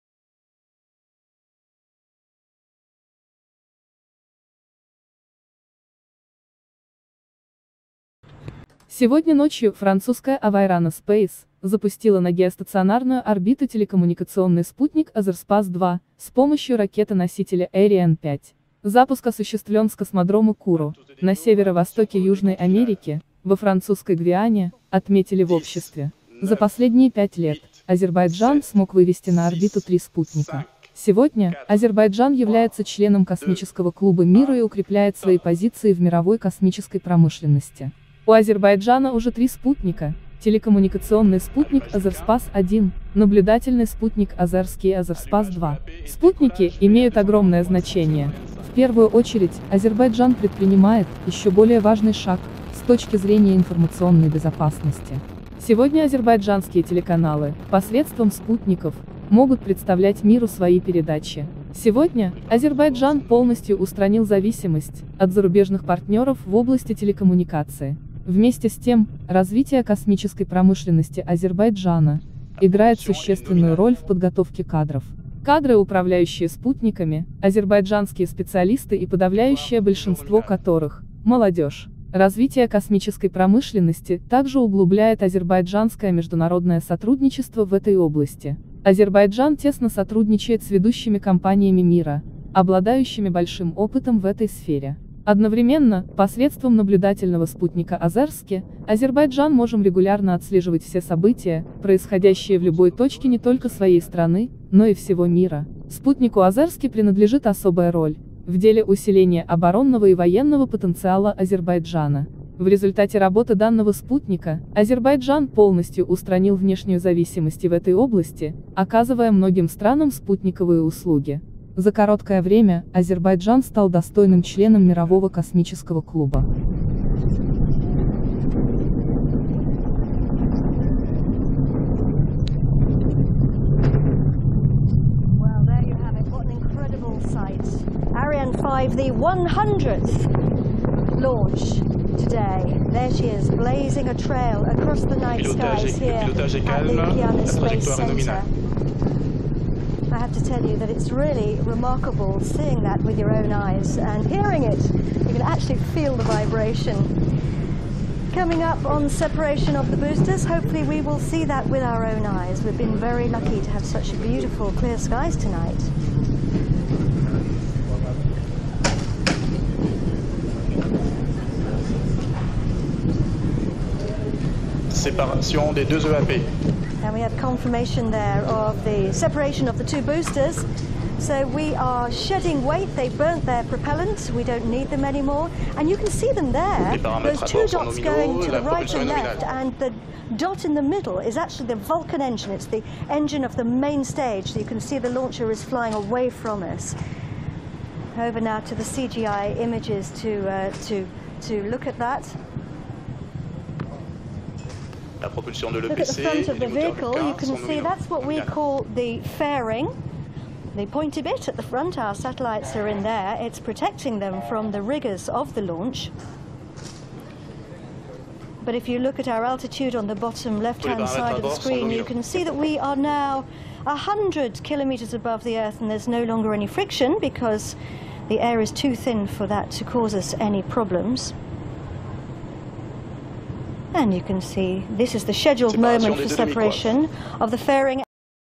Азербайджан запустил на орбиту третий спутник .
Азербайджанский спутник "Azerspace-2" был выведен на орбиту с космодрома Куру, находящегося во Французской Гвиане.